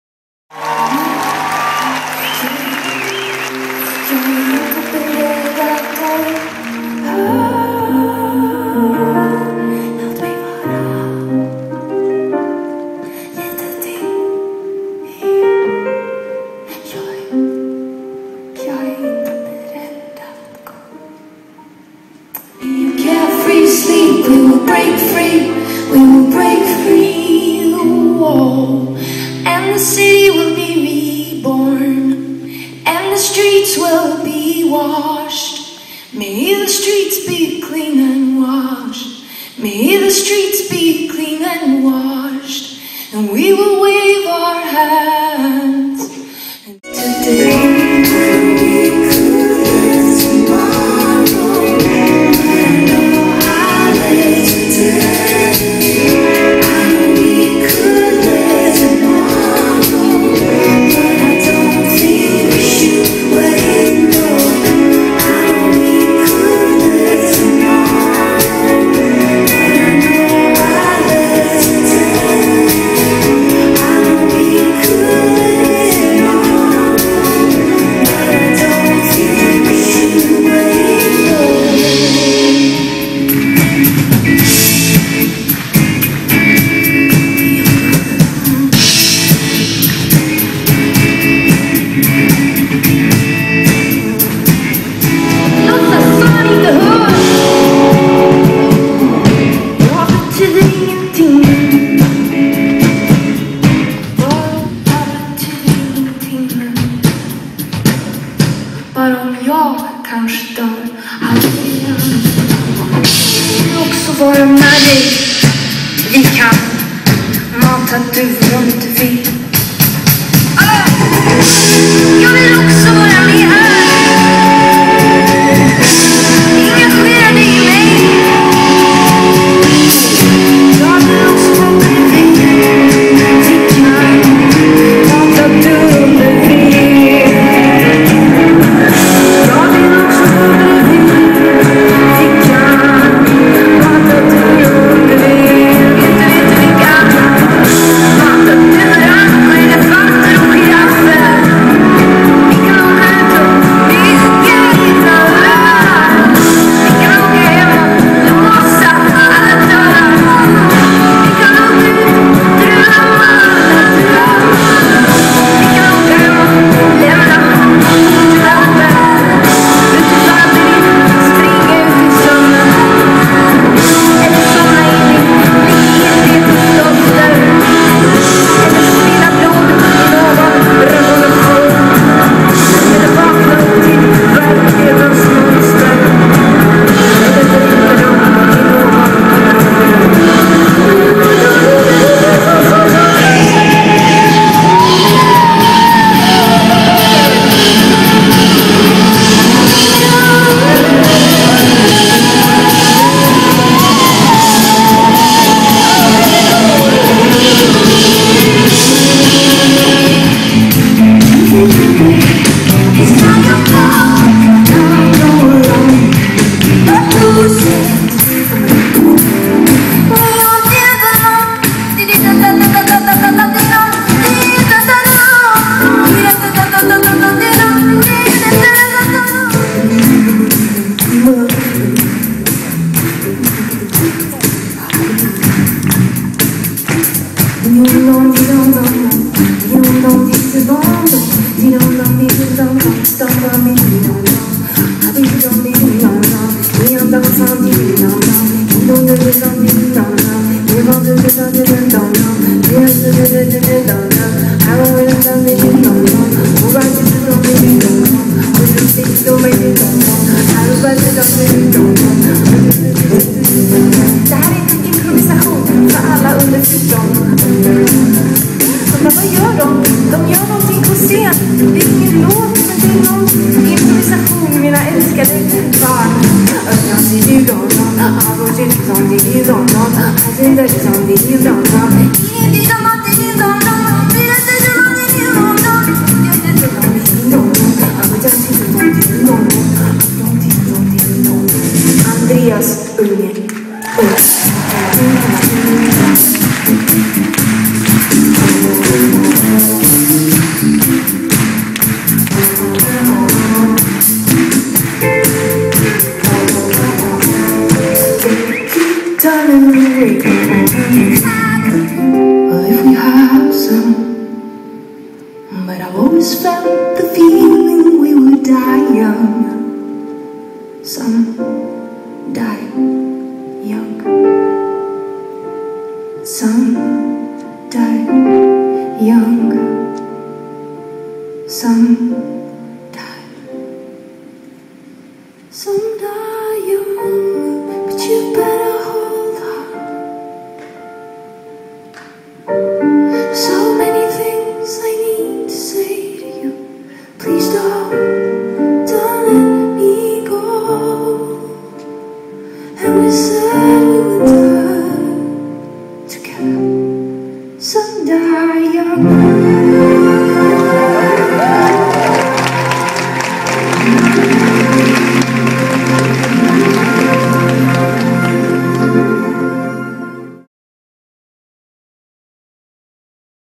Laleh på Cirkus April 2012
Korta mobiltelefonklipp från Lalehs konsert på Cirkus 17 april 2012